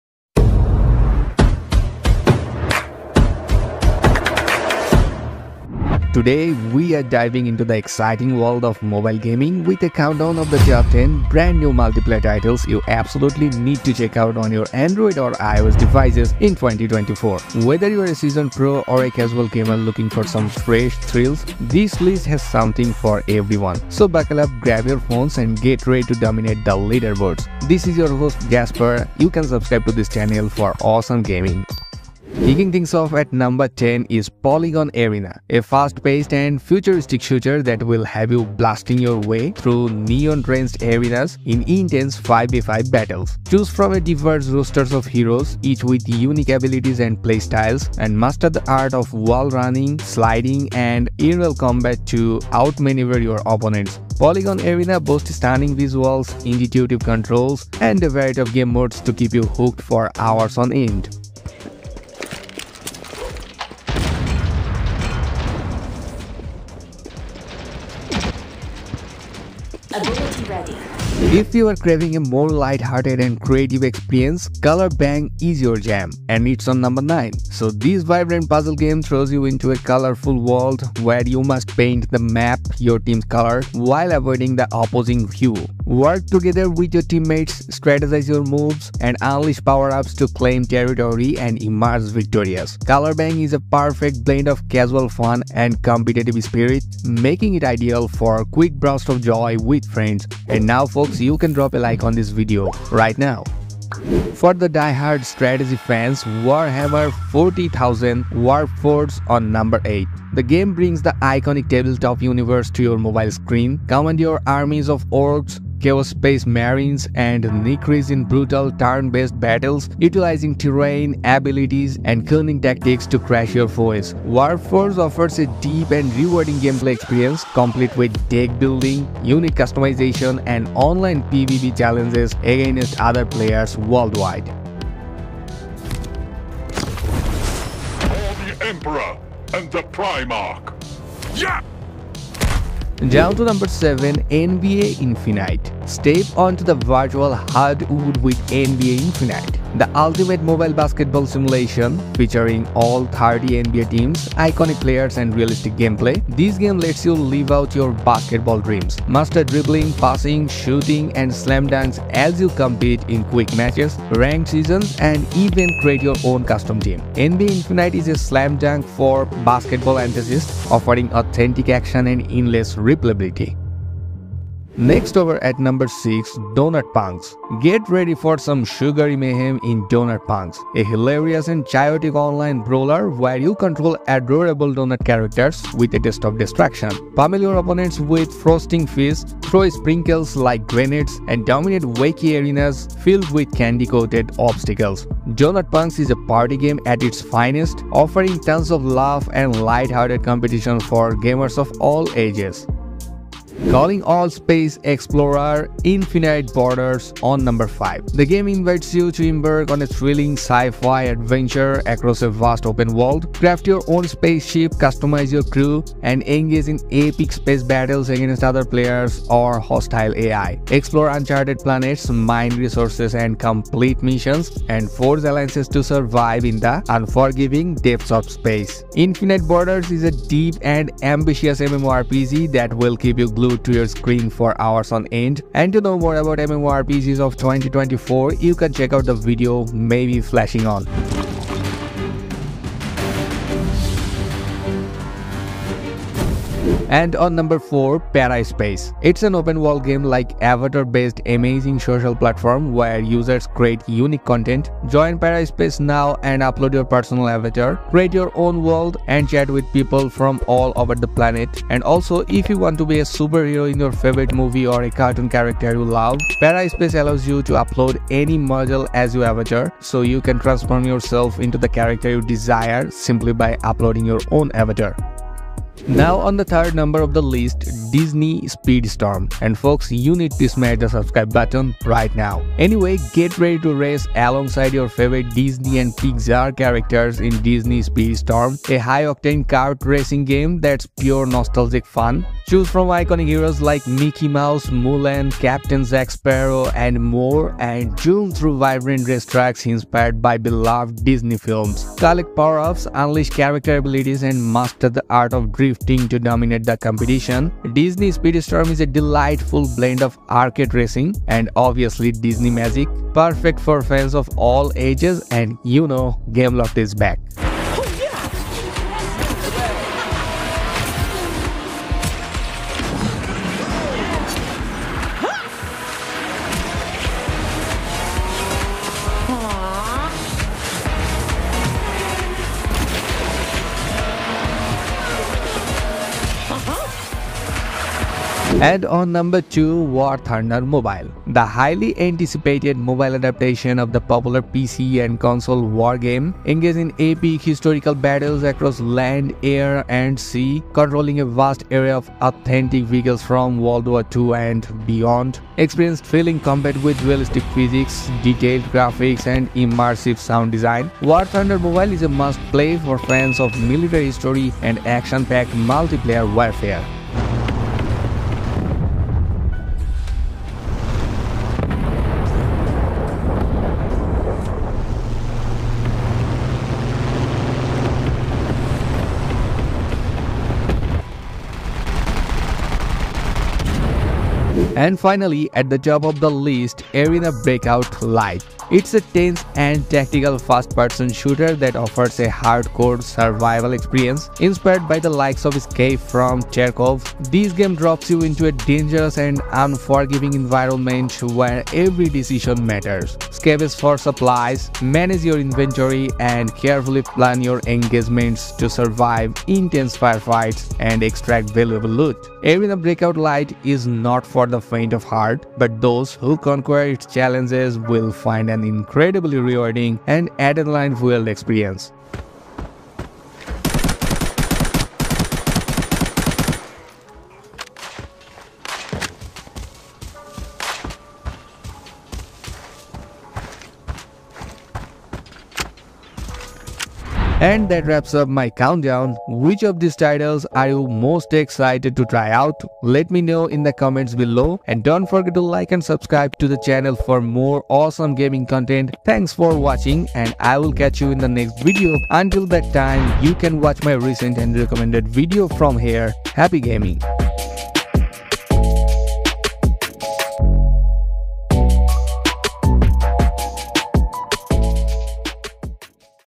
TOP 10 Best Multiplayer Games 2024 | New Android & iOS Games
This ain't your grandma's mobile gaming. We're talking fresh genres, mind-blowing graphics, and heart-pounding action that'll leave you glued to your screens. From cartoon chaos to tactical showdowns, this list has something for everyone.

Chapters:
00:00 Intro
00:32 Number 10
01:10 Number 9
01:44 Number 8
02:22 Number 7
02:54 Number 6
03:23 Number 5
04:06 Number 4
04:36 Number 3
05:38 Number 2
06:24 Number 1

Source: @Infraction & Free YT Music